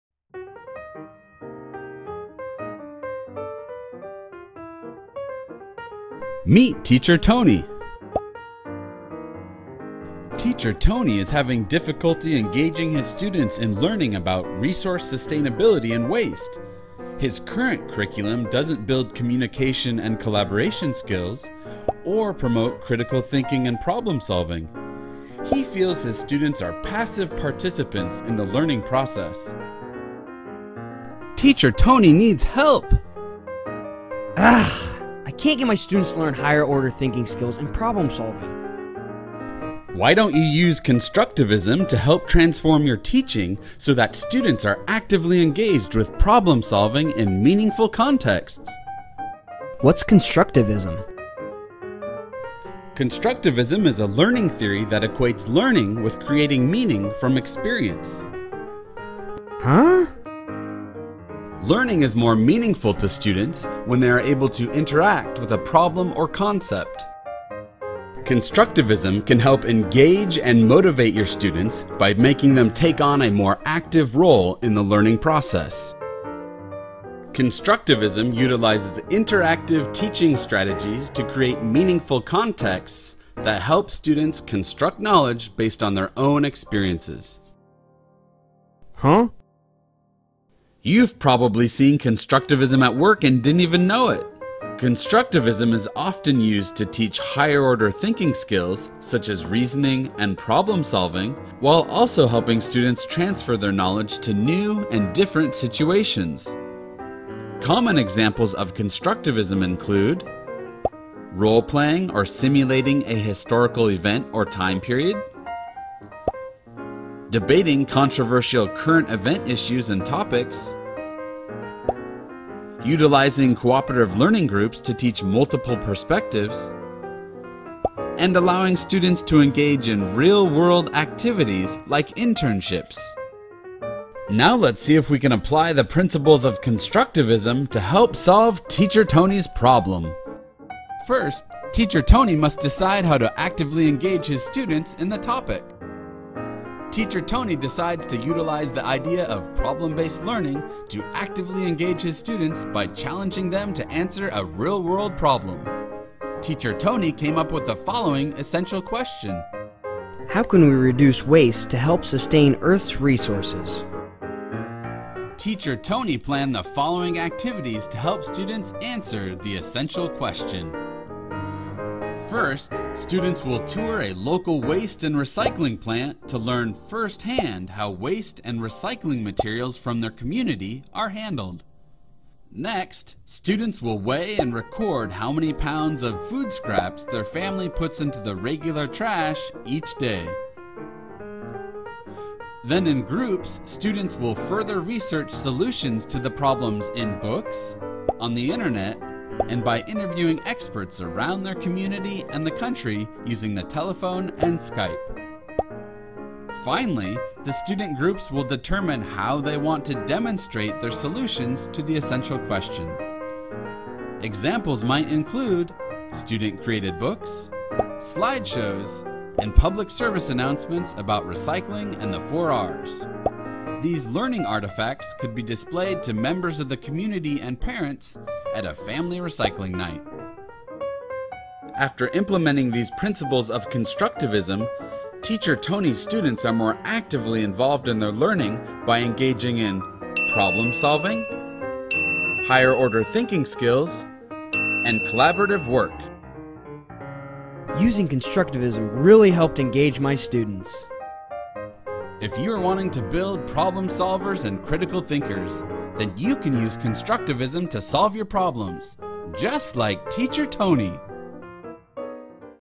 Use a Learning Theory: Constructivism
This short video explains the learning theory of constructivism. Watch this video to see how you can incorporate cognitivism into your classroom.